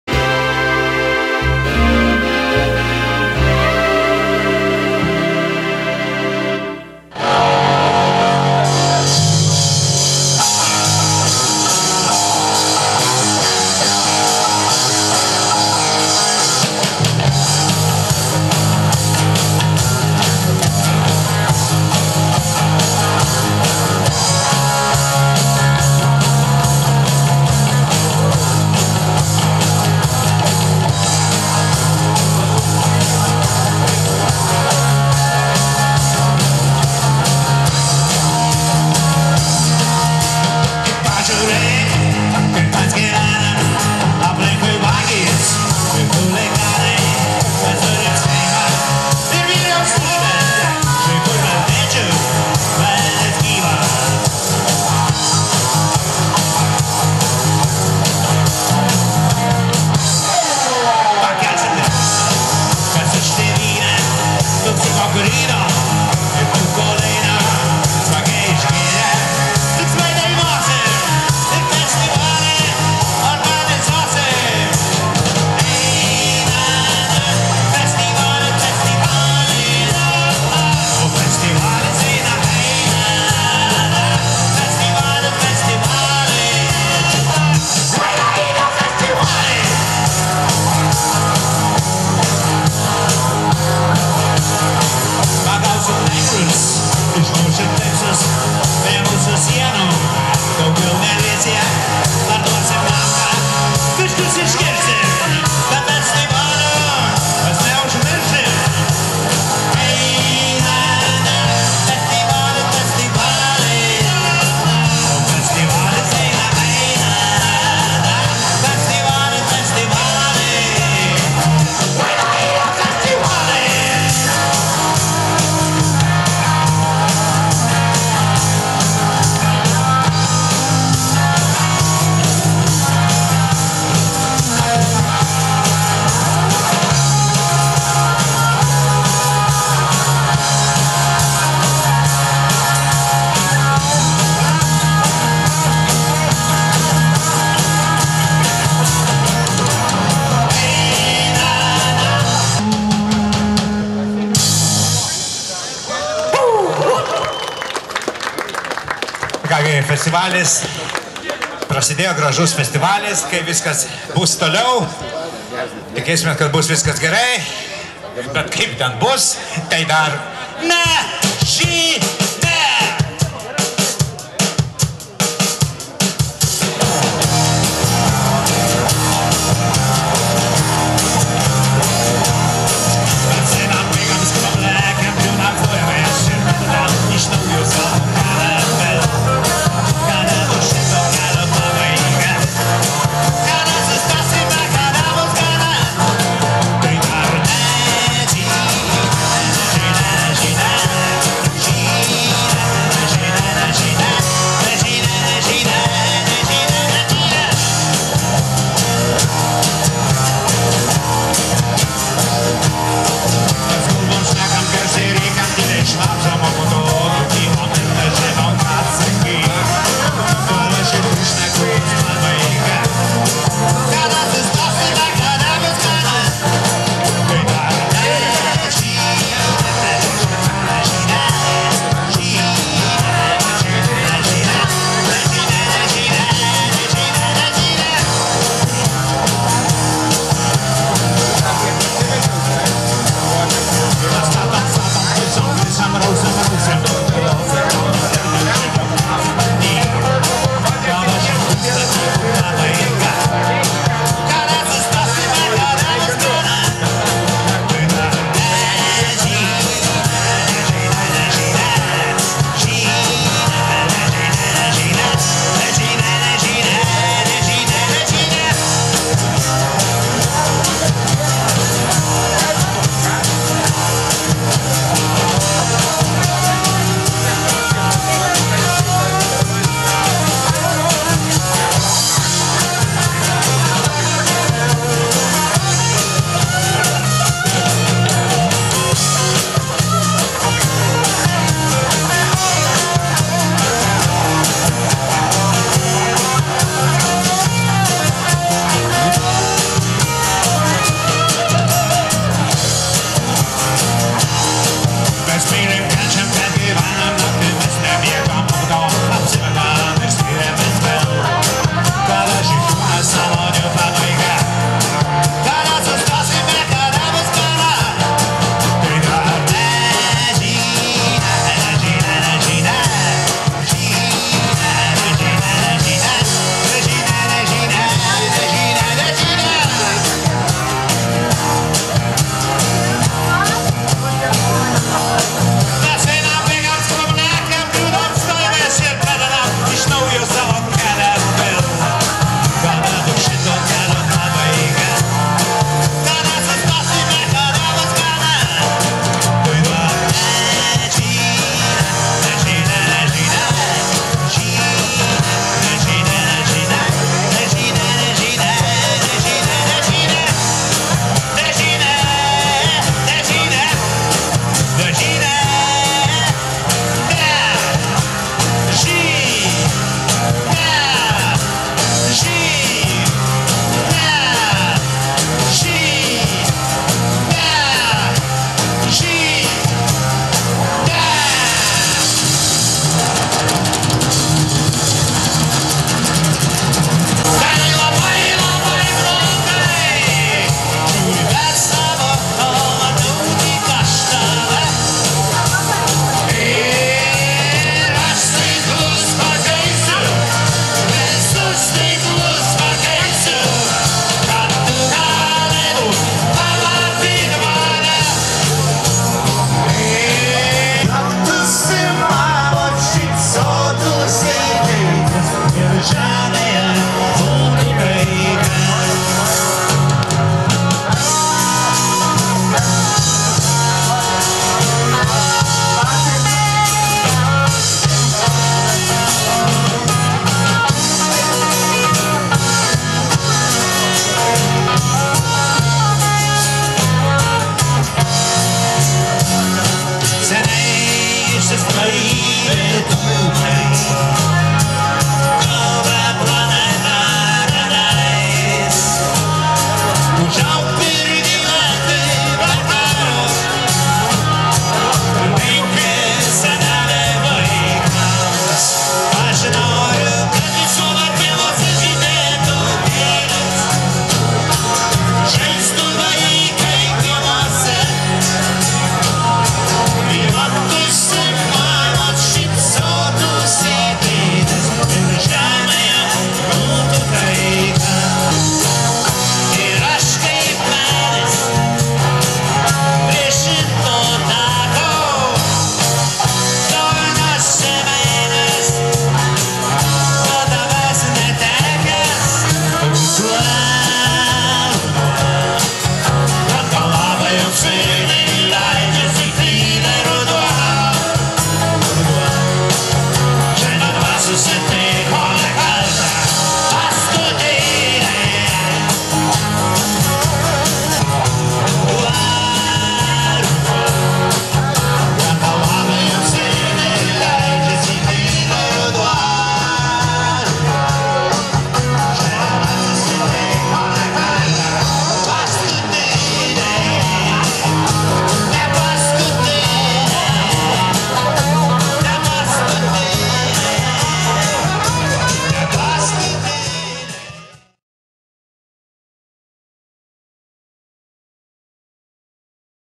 Šarūnas Mačiulis + VILKAI ir Jonas Vasilevičius (LT)
ORGANIZATORIAI: ALYTUS.IN ROCK, ROKO PIRTIS, SUTYLA LT ir Co.
BUNDA ROKAS 2015. Bundoriai. Nemuno kilpų regioninio parko teritorija.